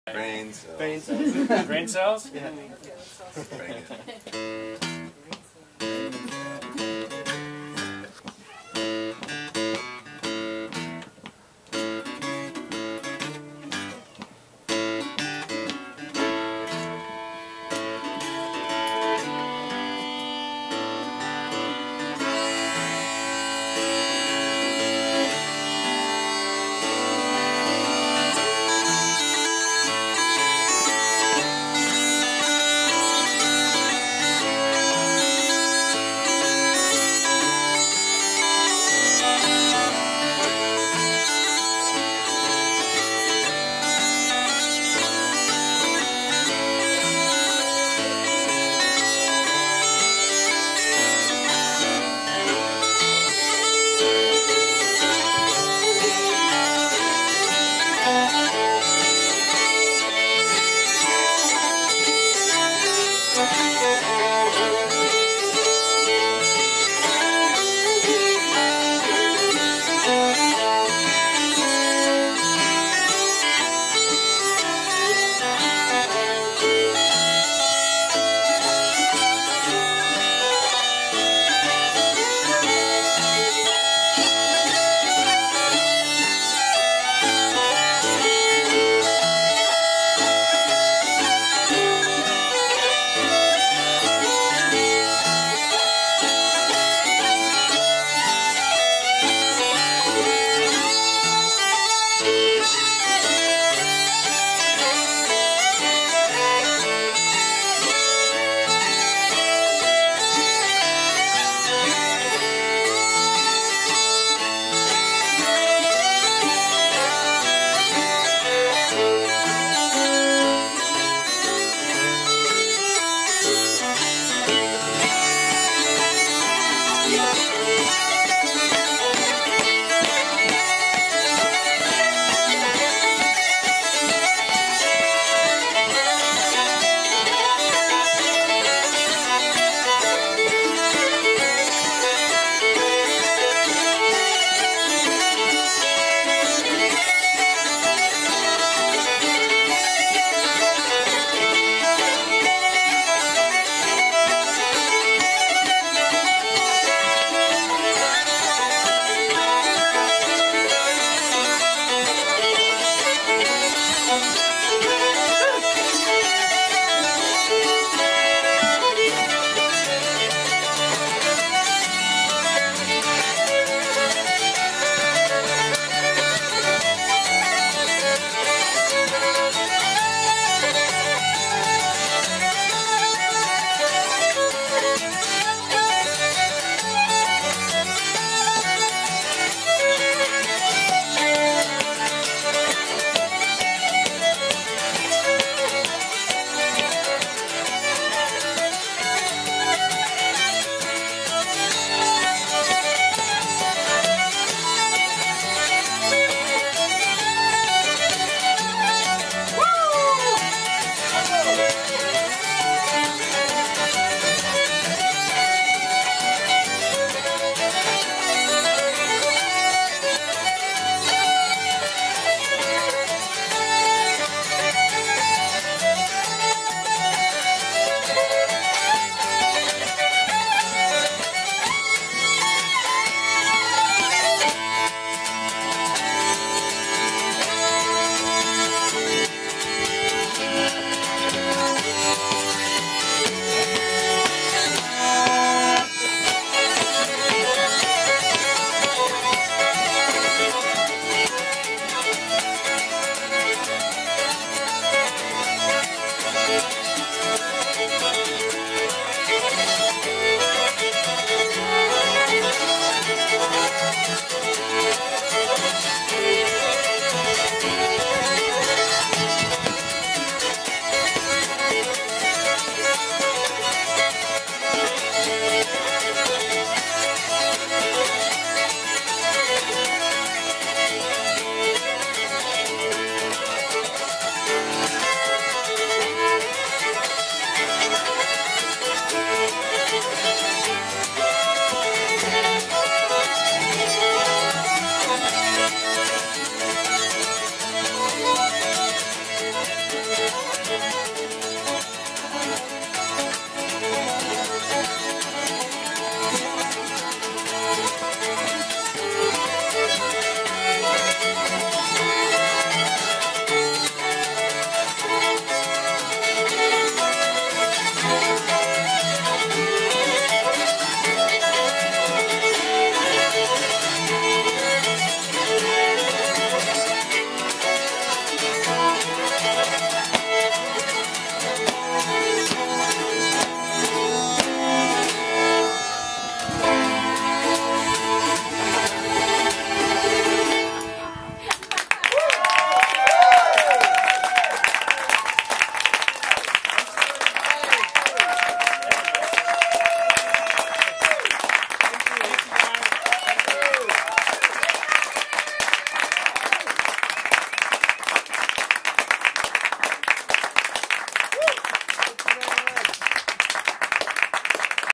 Molly's Revenge (Tree House Concerts 6/5/2011)
Molly's Revenge at Tree House Concerts in Carmel Valley Village, CA on Sunday, June 5, 2011. for more info.